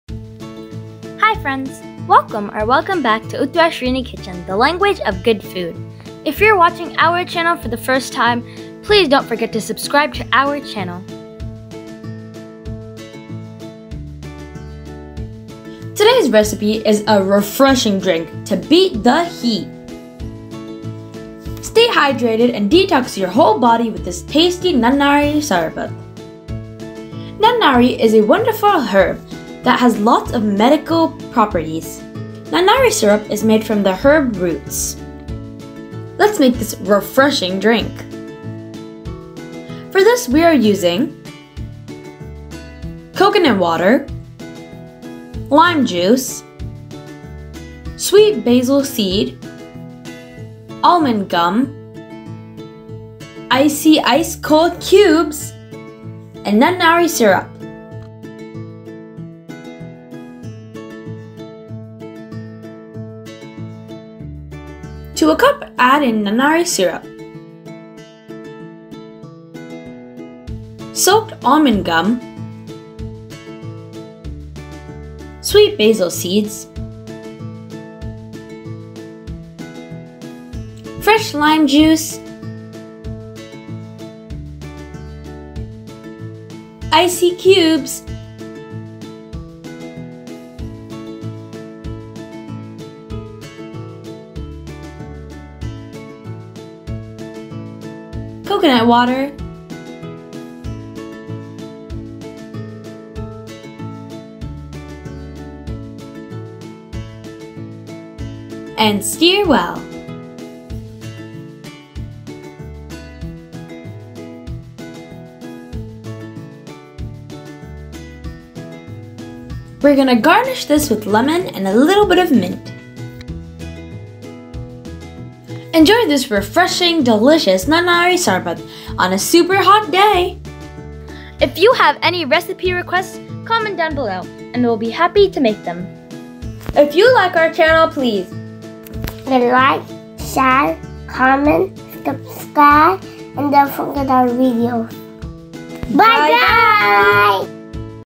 Nanari Sarbath | நன்னாரி சர்பத் | How to make Nannari Sarbath | Summer Special |నన్నారి షర్బత్
Welcome back to another video, today's recipe is a Traditional South Indian summer drink.
Nanari is wonderful herbal plant root, Nanari syrup acts as a body coolant and detox.
Staying hydrated and keeping the body cool is a must during the summer.
Try this wonderful Nanari sarbath on a hot summer day.

Also, share your feedback about the recipe in the comments below

Before you go, please visit my social profiles